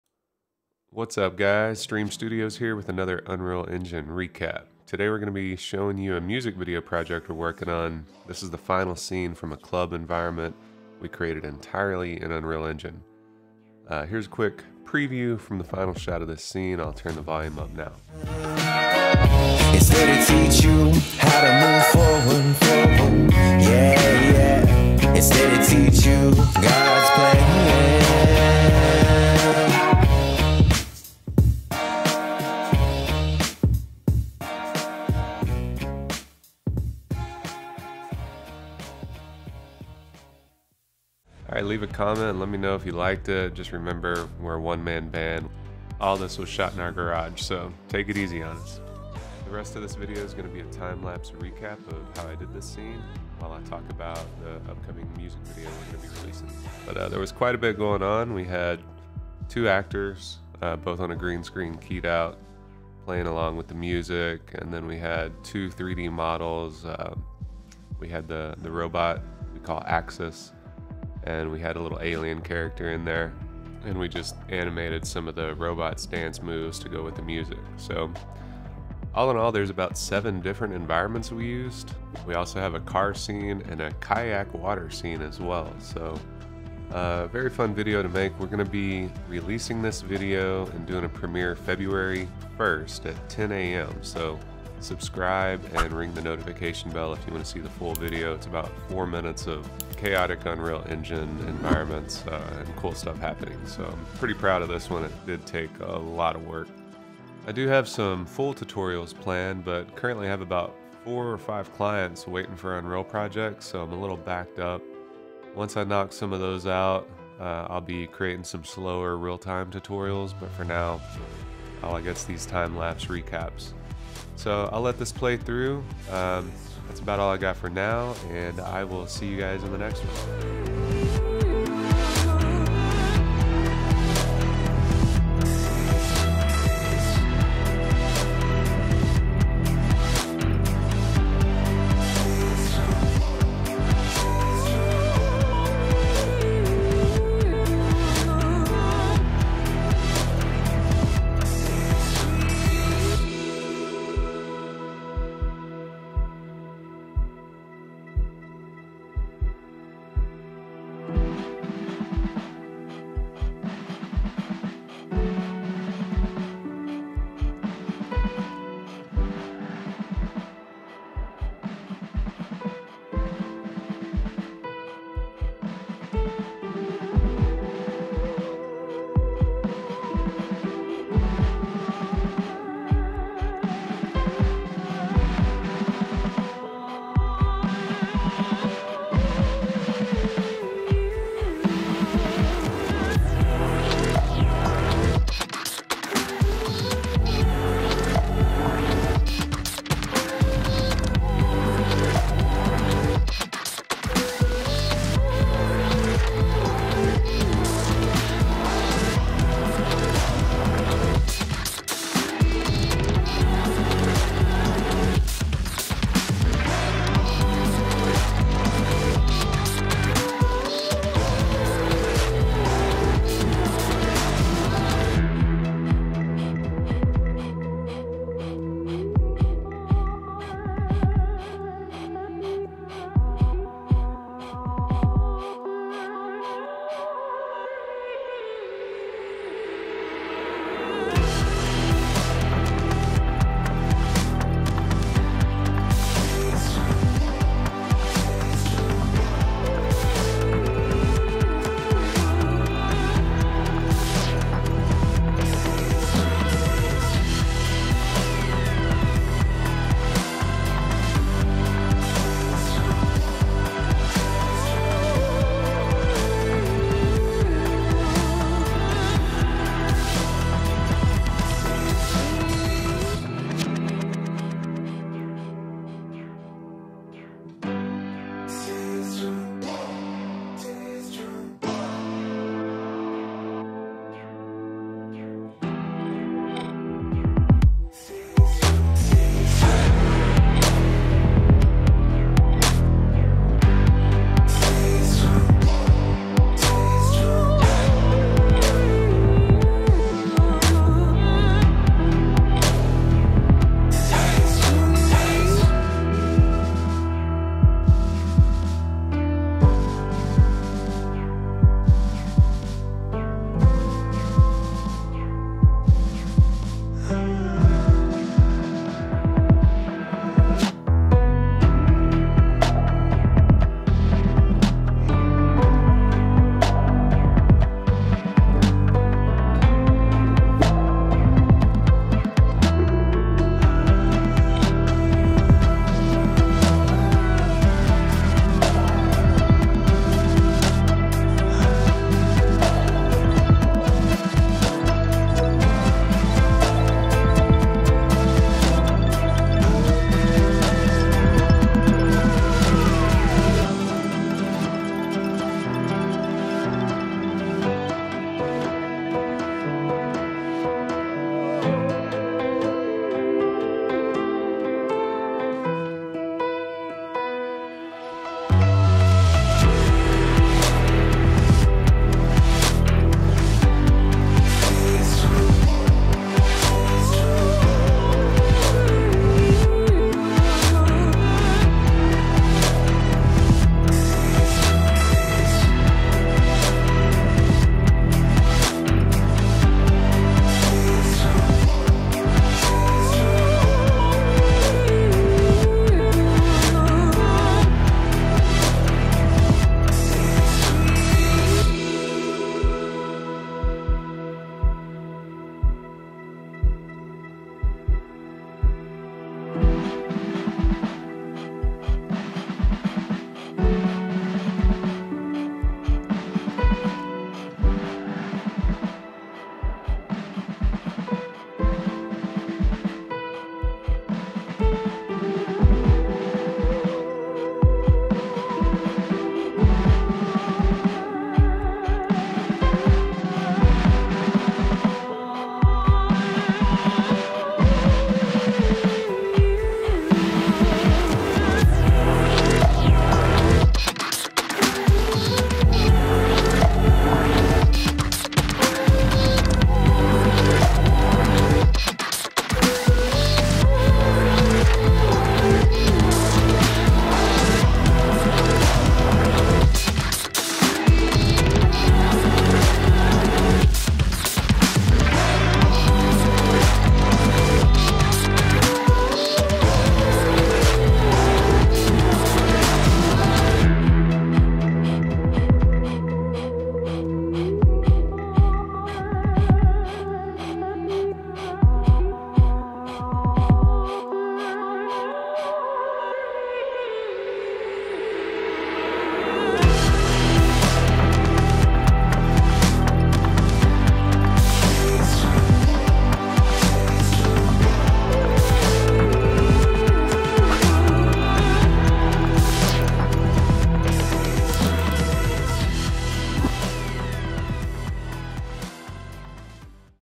Make Movies in your Garage using Unreal Engine 5.1 - Timelapse Recap
Unreal Engine can help you bring your music video or short film to life!

This is a timelapse recap showing how we brought this music video scene to life using unreal engine 5.1.

Longer real time tutorials coming soon.

Will be re-doing this render as there's some flicker on the bottles in the background, will be providing best render settings i find in a posted comment.

0:00 Intro
0:40 Timelapse Recap

Thanks!